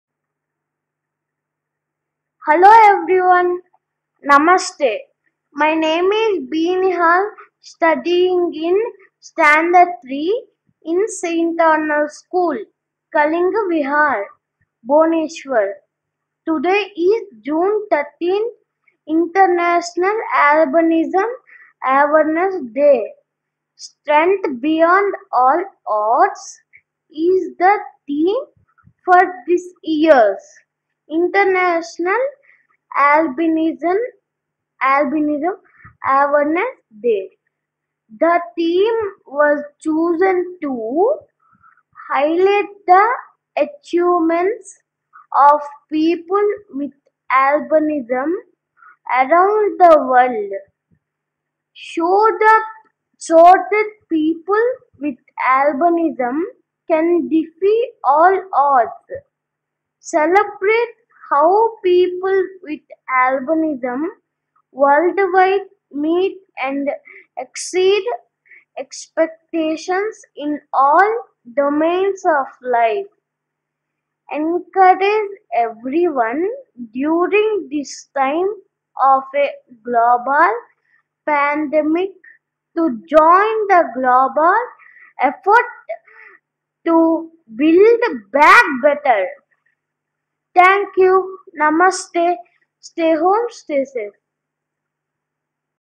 International Albinism Awareness Day 12th June 2021
International Albinism Awareness Day is held on June 13th every year to educate the public about albinism, a condition that is largely misunderstood. The United Nations launches educational campaigns on this day every year and typically invites people living with the condition to speak in front of the Assembly.

• Today is June 13: “International Albinism Awareness Day”
"Strength Beyond All Odds" is the theme for this year's
International Albinism Awareness Day.
The theme was chosen to:
• Highlight the achievements of people with albinism around the world.
• Show that people with albinism can defy all odds.
• Celebrate how people with albinism worldwide meet and exceed expectations in all domains of life.
• Encourage everyone during this time of a global pandemic to join the global effort to Build Back Better